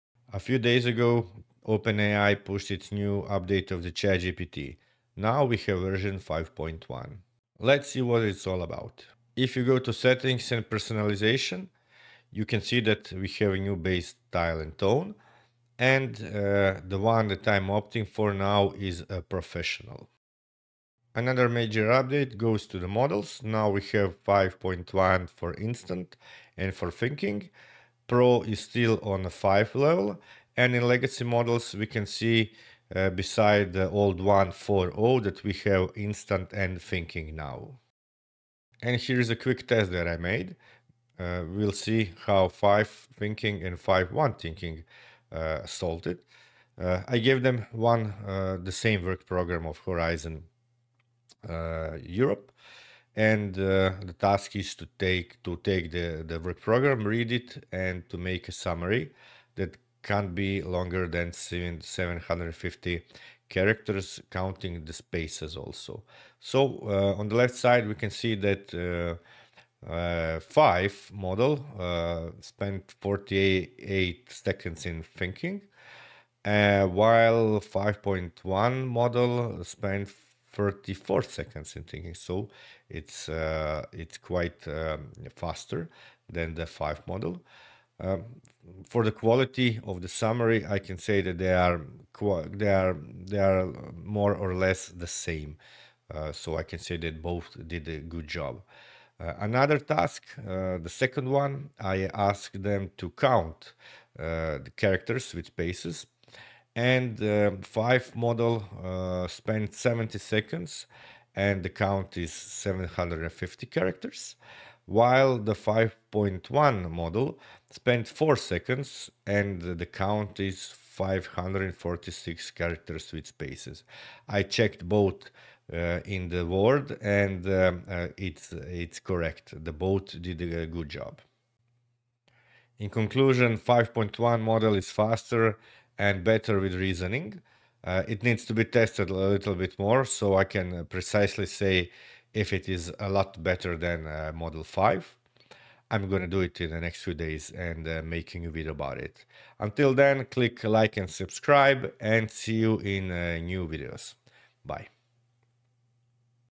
ChatGPT 5.1 is here. How much better is it than 5
OpenAI has just rolled out the new ChatGPT 5.1 update and in this video I run a real world test using an actual Horizon Europe Work Programme.

In this video you will see:
- where to find the new Settings and Personalization options
- what changed in the base style and tone and why I chose the professional one
- what is new in the model list for Instant and Thinking
- a practical test on a real Horizon Europe Work Programme document
- a comparison of thinking time and summary quality between 5 and 5.1
- a character count test with spaces and how accurate both models are

In my test, the 5.1 Thinking model was noticeably faster, while the quality of the summaries was very similar to version 5. Both did a solid job, but 5.1 also showed better reasoning and faster character counting.

This is just an initial test. I will run more detailed experiments on complex tasks related to EU funding and Horizon Europe project preparation and share the results in future videos.

If you are using AI tools, working with EU funds or just want to get more out of ChatGPT for serious work
- comment what kind of tests you want to see next